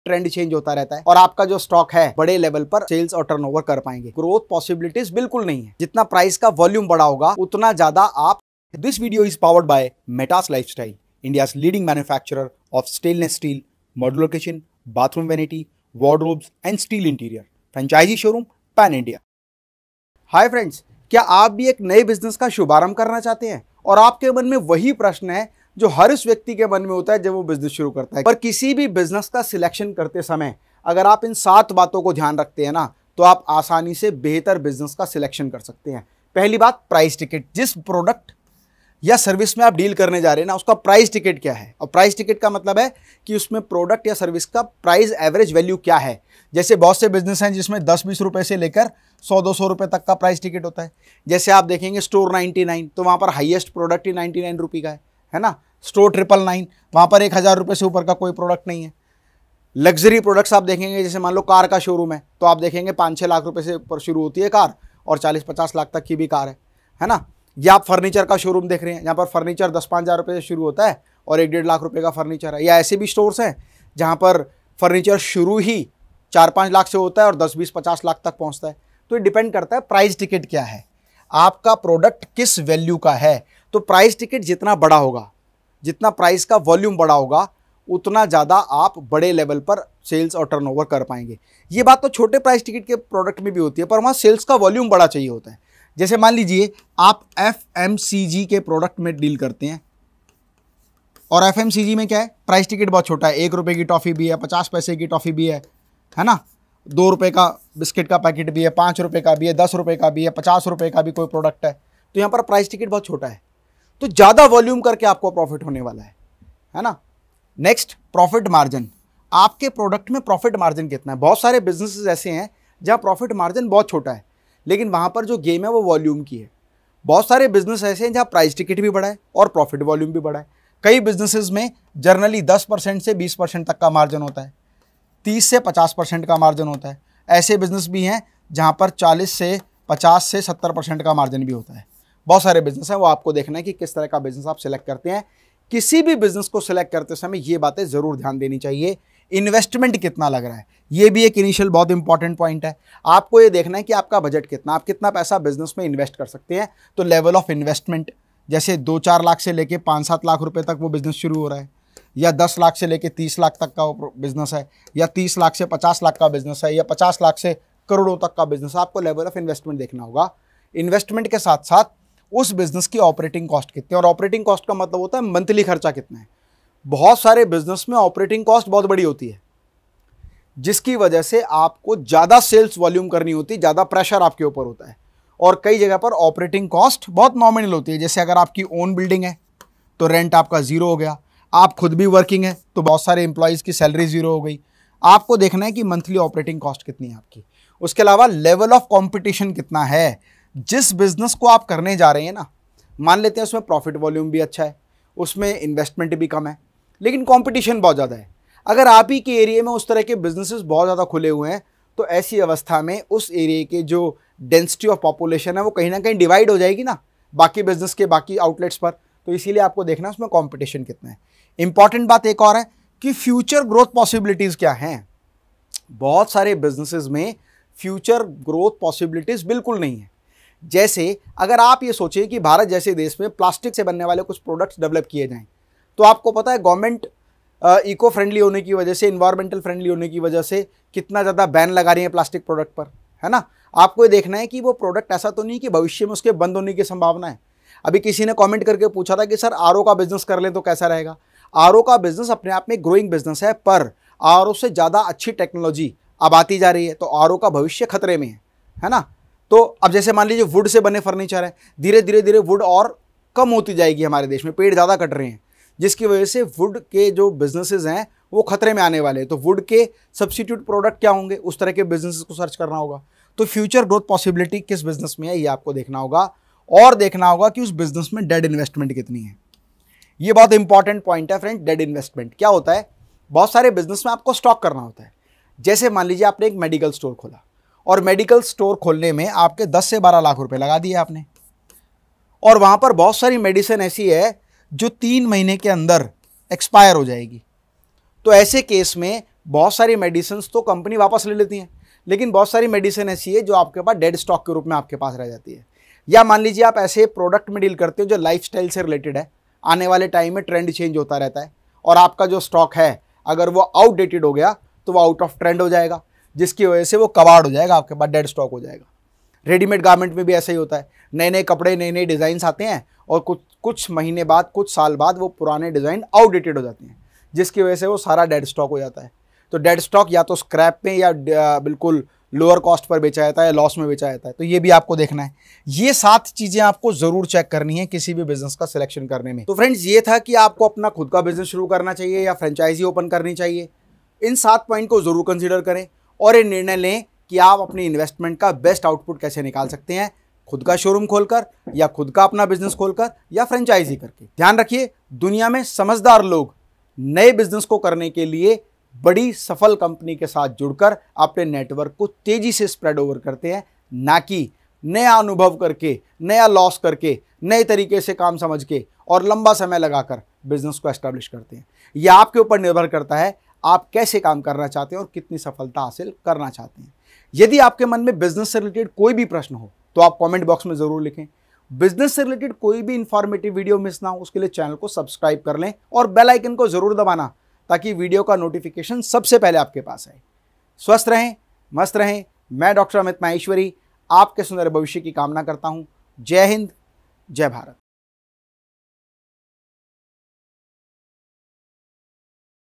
How To Select Right Business | सही व्यापार कैसे चुनें | Dr. Amit Maheshwari
In This Video Dr. Amit Maheshwari Sir Is Guiding You About How You Can Select the Right Business.